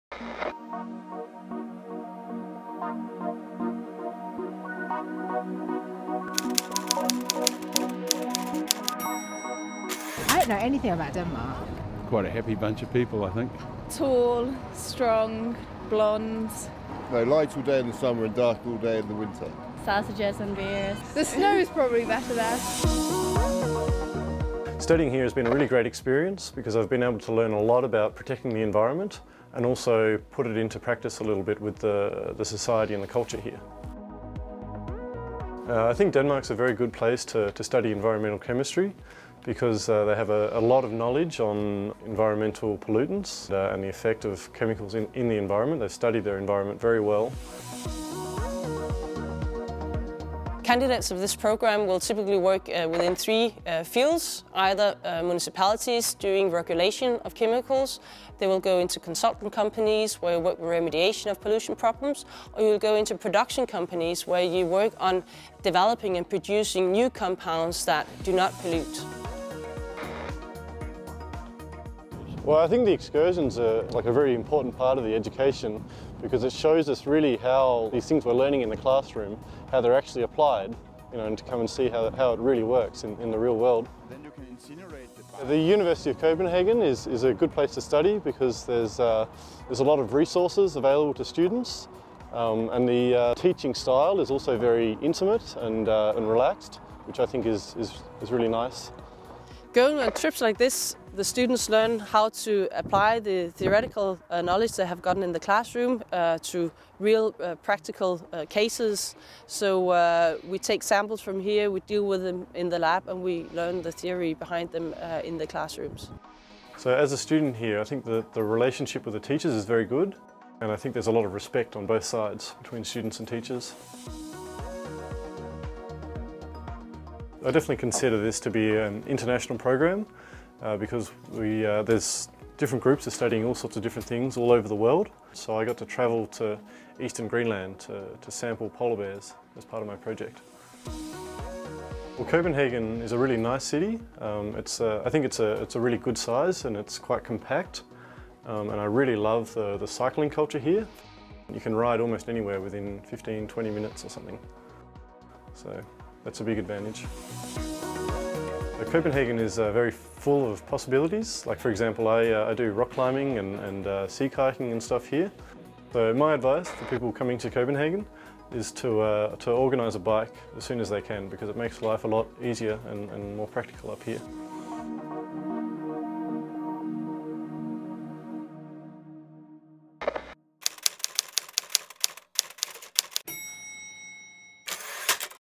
Study Environmental Sciences at the University of Copenhagen
Meet an international student and the Head of Studies of the Environmental Sciences MSc programme at the University of Copenhagen, Denmark.